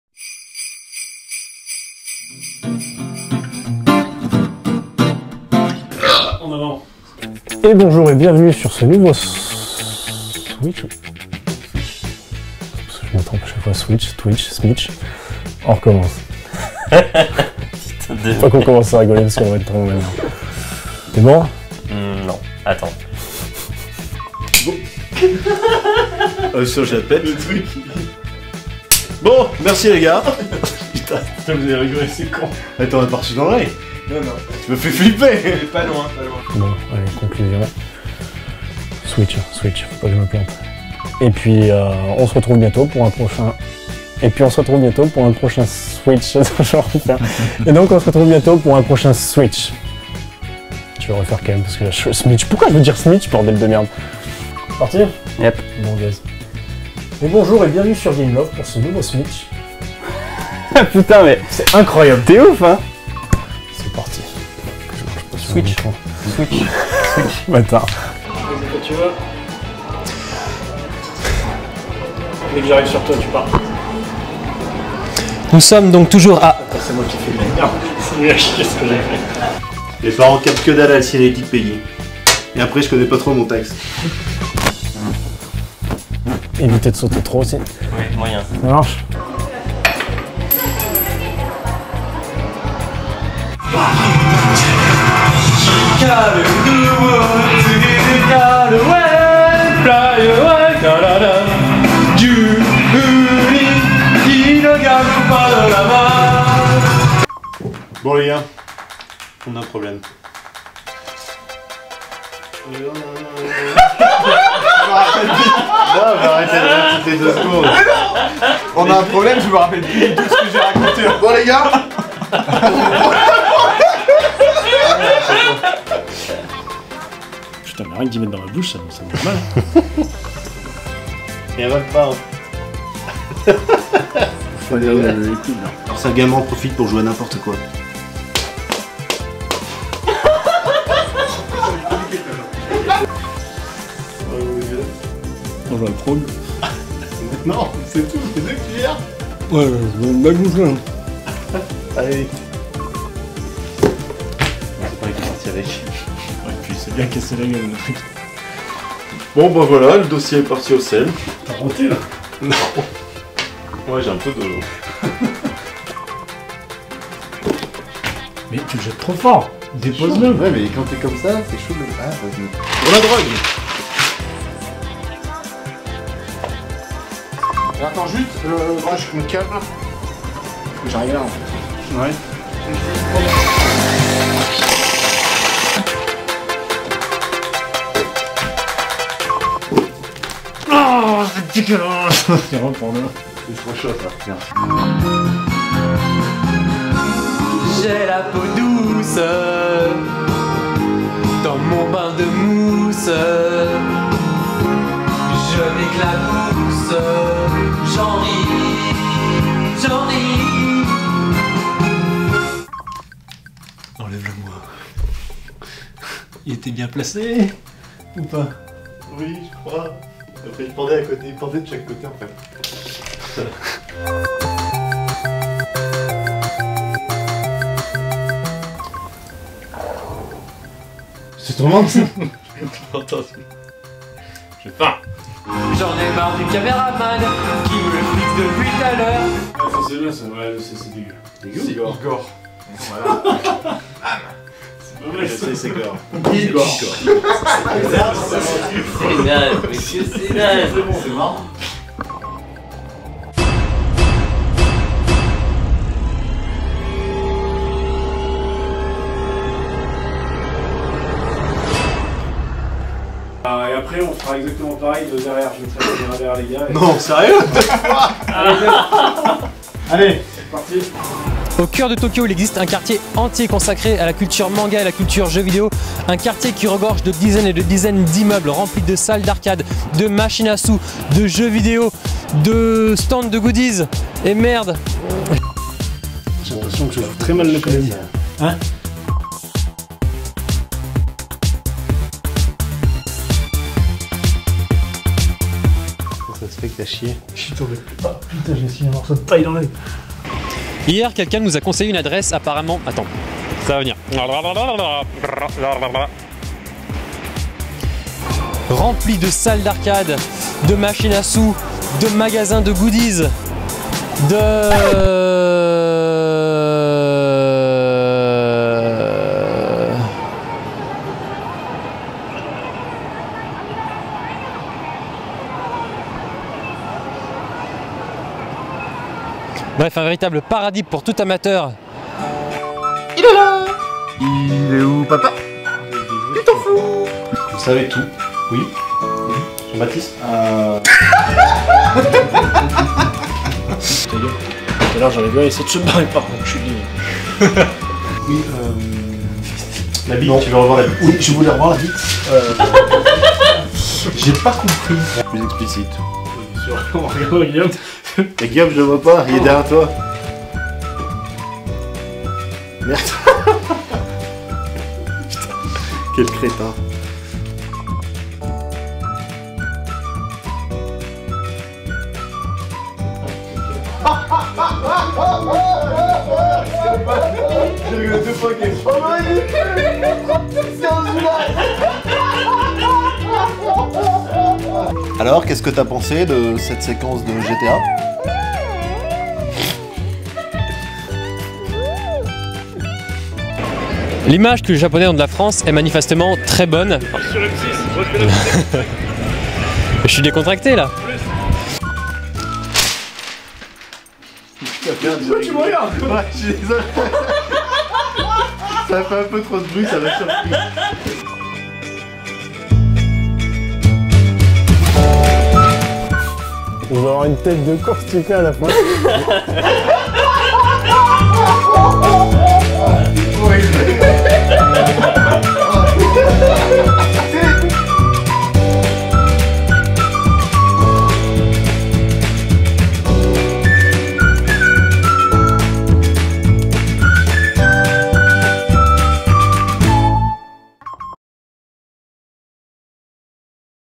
Gamelove.com : Le Bêtisier de Noël 2014
Lancé en avril de cette année, toute l'équipe de Gamelove tenait à vous remercier en vous proposant un petit bêtisier comme le veux la coutume en période de fêtes de fin d'année. L'occasion de retrouver pas mal de têtes parmi l'équipe. l'occasion également de vous moquer de nous allègrement. Dans tous les cas, merci infiniment du soutien que vous nous témoignez. Le lancement d'un nouveau média n'est jamais quelque chose de simple mais tous les retours positifs que vous nous envoyez parviennent à nous motiver suffisamment pour resigner en 2015. L'objectif ? Plus de vidéos, de concept, quelques tentatives de directs, plus de qualité aussi et un amené à évoluer en terme de contenus comme en rubriques, technique et tout un tas de truc en "ique".